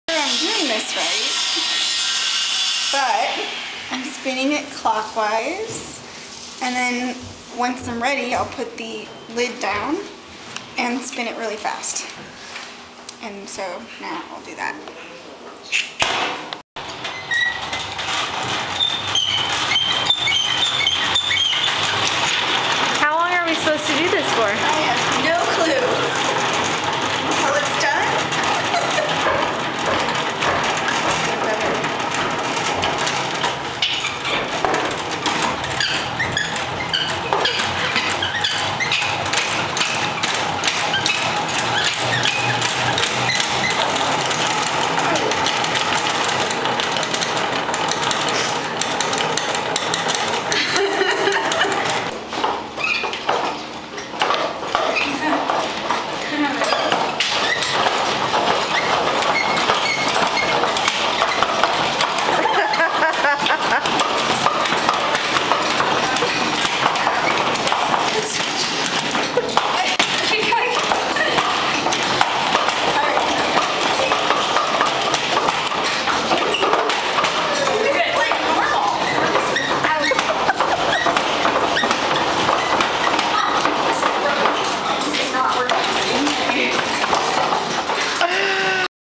Extracting honey with Kimberley and Bree at Sunset Magazine HQ
Typically when we harvest honey here at Sunset Magazine HQ, we strip the wax  comb and honey from the frames and strain the honey through cheesecloth into a bowl.  This time we thought we would be a little more daring and use a honey extractor (available for use though our local bee guild).  The honey extractor uses centrifugal force to fling the honey out of the comb.  Using this system allowed us to keep most of the comb intact so we would be able to put the frames back in the hive and the bees would not have to rebuild all of the comb before filling it with new brood or honey.